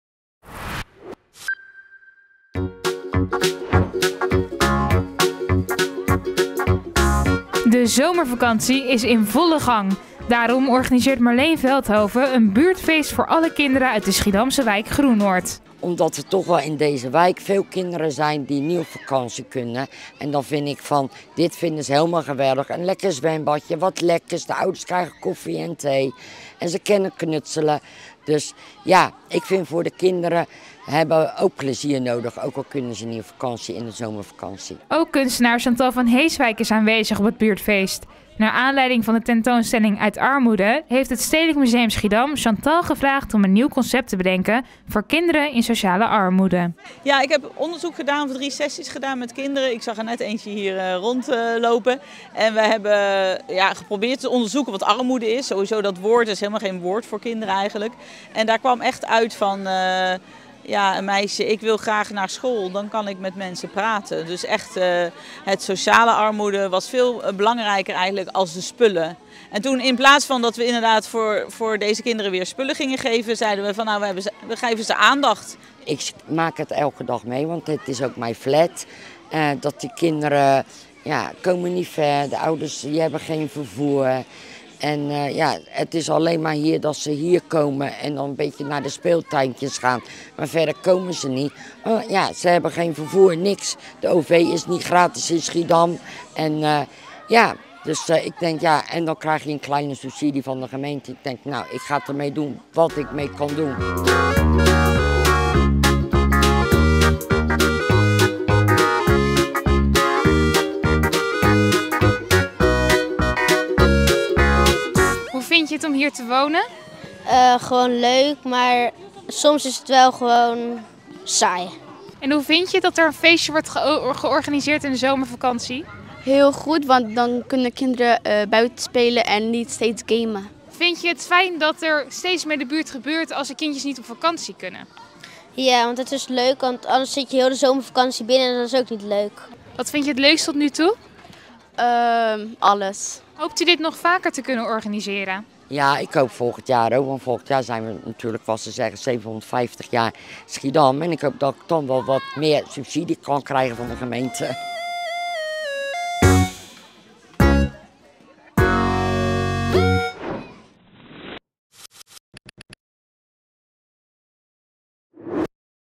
Chantal en Marleen organiseren buurtfeest tegen eenzaamheid in de zomervakantie
De zomervakantie is in volle gang! Daarom organiseert Marleen Veldhoven een buurtfeest in Schiedam Groenoord, zodat kinderen die thuisblijven ook van de vakantie kunnen genieten en zich niet eenzaam voelen. Het Stedelijk Museum Schiedam ondersteunt dit initiatief door een tent met knutselspullen op te zetten, waar kinderen samen met museummedewerkers kunnen knutselen.